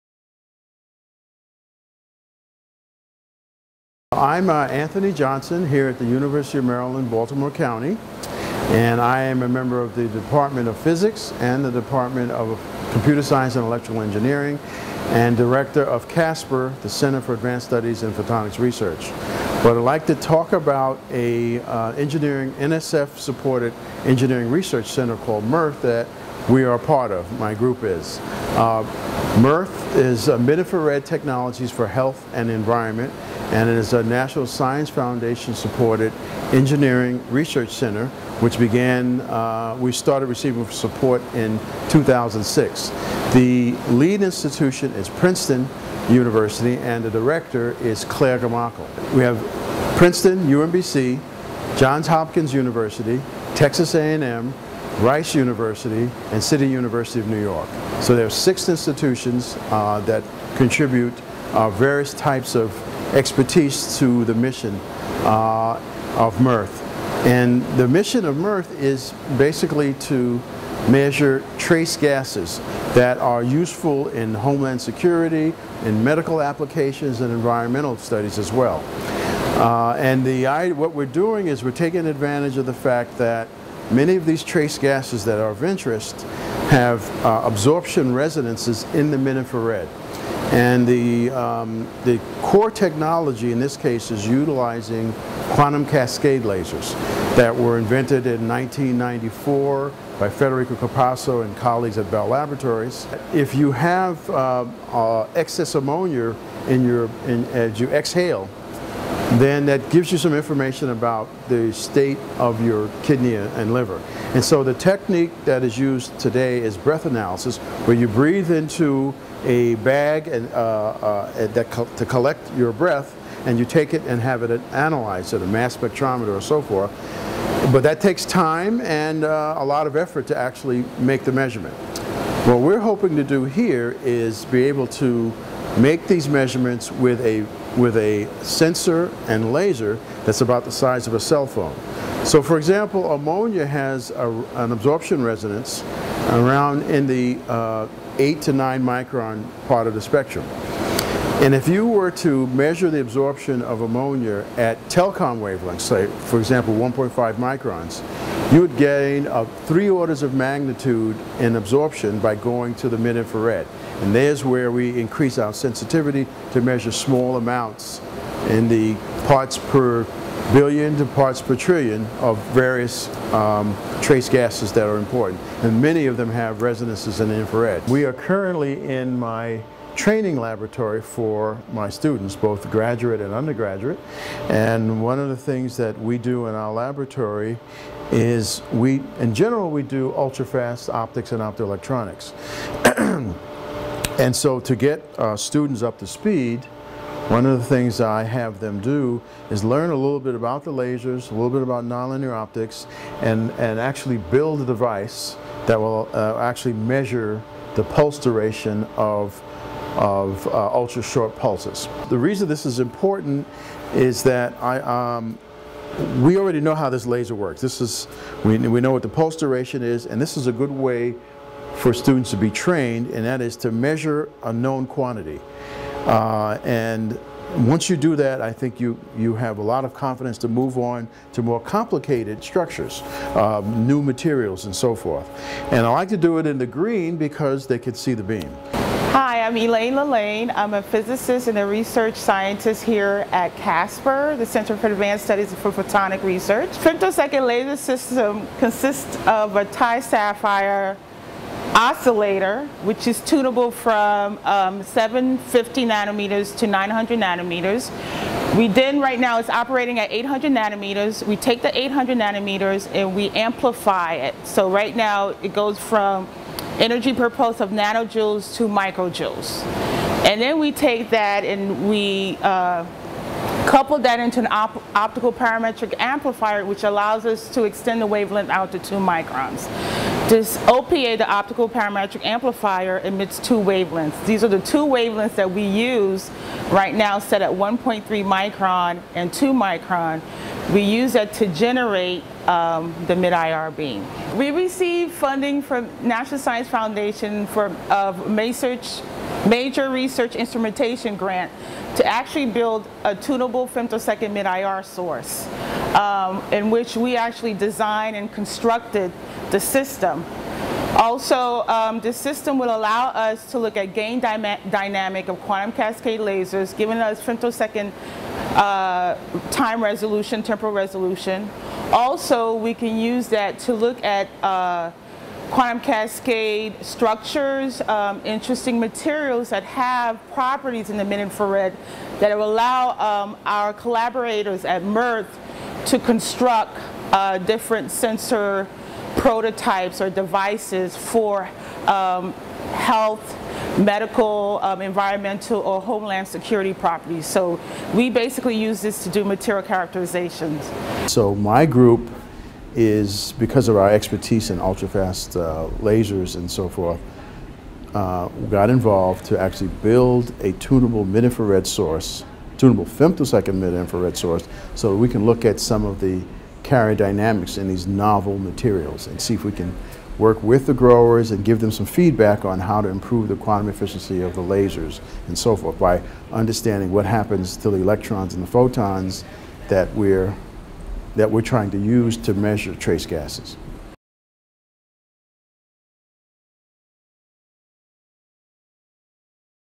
Anthony Johnson: CASPR explores the mid-IR for new sensing capabilities
Mid-infrared sensors have the ability to detect minute amounts of chemicals in the environment or atmosphere, exhaled by humans, or emitted from spills, combustion, or natural sources.

- SPIE Defense Security + Sensing Symposium

Anthony Johnson is Director of the Center for Advanced Studies in Photonics Research (CASPR) and professor of Physics and Computer Science & Electrical Engineering at the University of Maryland, Baltimore County (UMBC). He is also a director of MIRTHE (Mid-Infrared Technologies for Health and the Environment).

MIRTHE is a National Science Foundation Engineering Research Center headquartered at Princeton University, with partners City College New York, Johns Hopkins University, Rice, Texas A&M, and the University of Maryland Baltimore County. The center encompasses a world-class team of engineers, chemists, physicists, environmental and bio-engineers, and clinicians. MIRTHE's goal is to develop Mid-Infrared (£ - 3-30 µm) optical trace gas sensing systems based on new technologies such as quantum cascade lasers or quartz enhanced photo-acoustic spectroscopy, with the ability to detect minute amounts of chemicals found in the environment or atmosphere, emitted from spills, combustion, or natural sources, or exhaled.

Johnson graduated with a BS in Physics from the Polytechnic Institute of New York in 1975 and earned a PhD in Physics in 1981 from the City College of the City University of New York (CCNY). His PhD thesis research was conducted at AT&T Bell Laboratories in Murray Hill, NJ, with support from the Bell Labs Cooperative Research Fellowship Program for Minorities. He was a Distinguished Member of the Technical Staff in the Photonic Circuits Research Department at Bell Labs for 14 years prior to joining the New Jersey Institute of Technology in 1995, where he was Chairperson and Distinguished Professor of Physics until 2003.

His current research interests include the ultrafast photophysics and nonlinear optical properties of bulk, nanostructured, and quantum well semiconductor structures, ultrashort pulse propagation in fibers and high-speed lightwave systems. Johnson received the 1988 AT&T Bell Labs Distinguished Staff Award; the 1994 Black Engineer of the Year Special Recognition Award; the 1996 Edward A. Bouchet Award of the APS; the 2005 Science Spectrum Magazine Trailblazer Top Minority in Science Award; and an Honorary Doctor of Science degree from CCNY in 2011.

Elaine Lalanne received a BA in Physics from Wellesley College in 1994 and a PhD from the joint department of Applied Physics from New Jersey Institute of Technology/ Rutgers University-Newark in May 2003. She is currently a Research Scientist at CASPR, where her work involves studying the nonlinear optical properties of bulk, nanoclustered, and quantum-well semiconductor structures. She co-developed an integrated laboratory mid-IR ultrafast diagnostic instrument. Her current research focuses on four-wave mixing in chalcogenide waveguide in the mid-IR and investigating gain dynamics in quantum cascade lasers and structures.

The work described in the video was partially supported by NSF Major Research Instrumentation Grant ECS-0619548 and NSF MIRTHE Engineering Research Center Grant ERC-0540832.